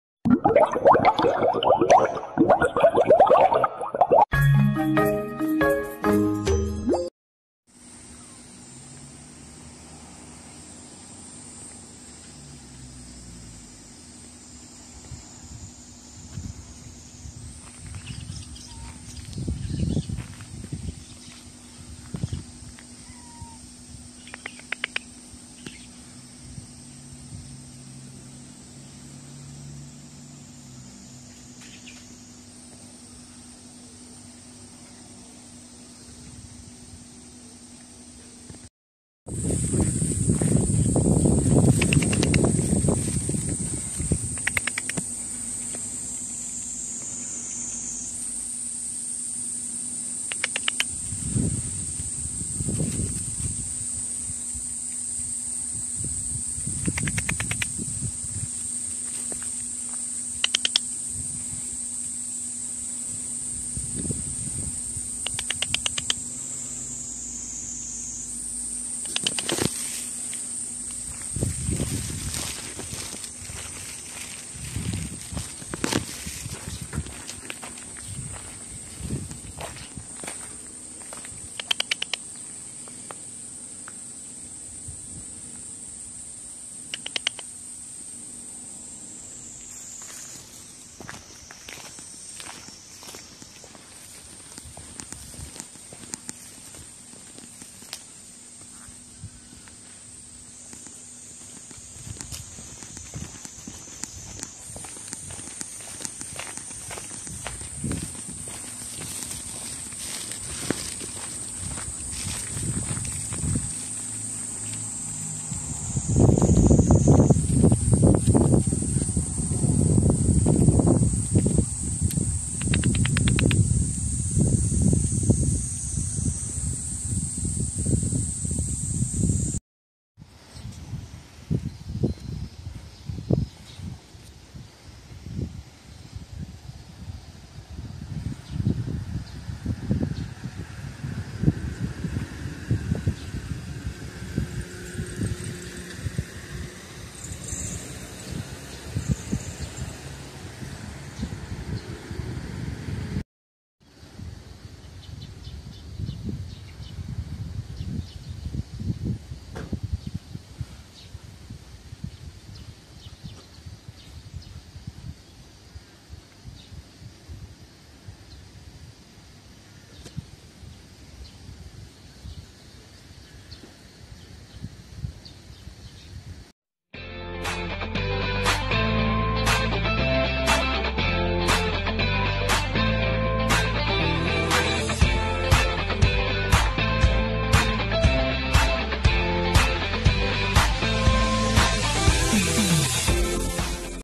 【朗報】廃屋の野良にゃんに赤ちゃん猫が🐱 成長したカモのヒナ🐤 20180730、野良猫、カモ
【朗報】廃屋の野良にゃんに赤ちゃんが🐱  成長したカモのヒナ🐤 20180730、野良猫、カモ